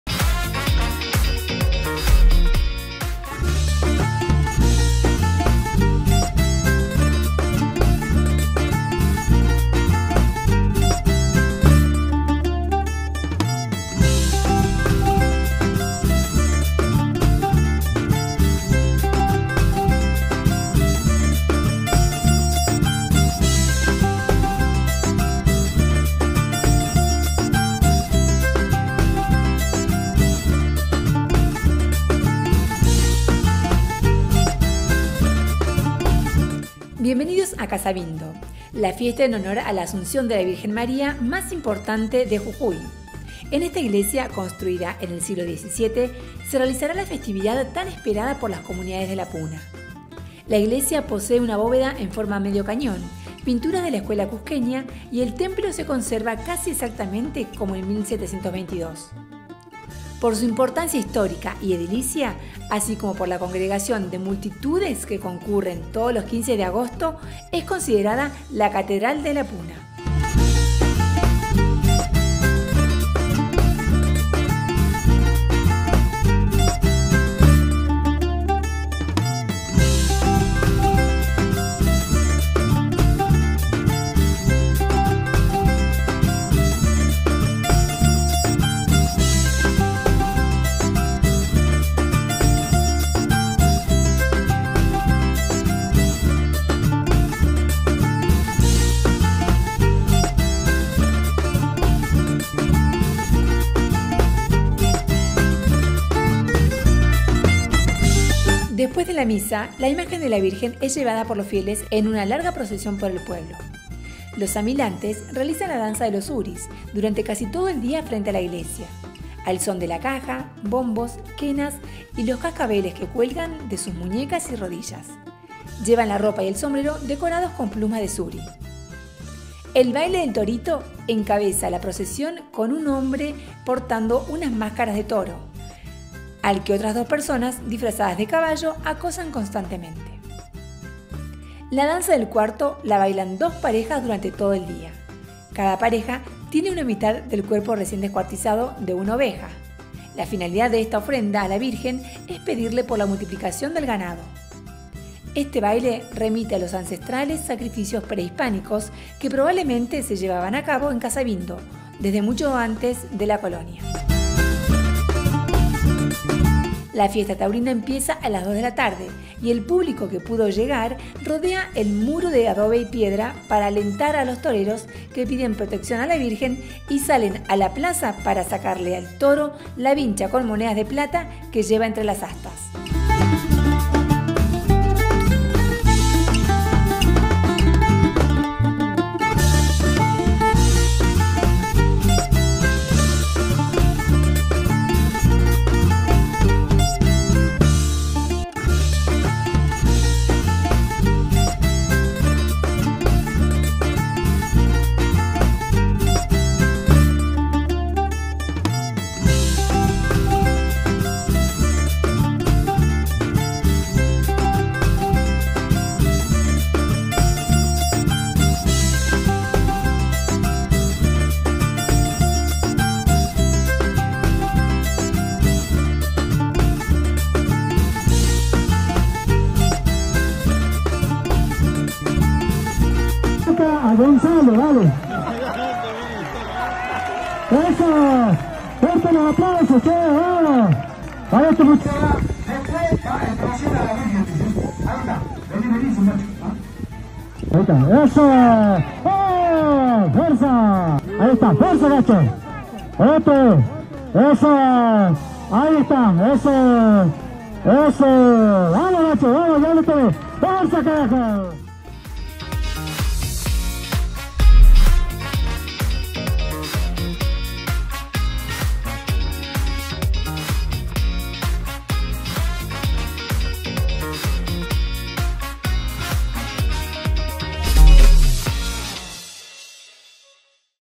Fiesta en Casabindo El toreo de la Vincha
Estuvimos en la tradicional Fiesta de la Virgen de la Asunción en Casabindo. Un pueblo ubicado en la Puna jujeña.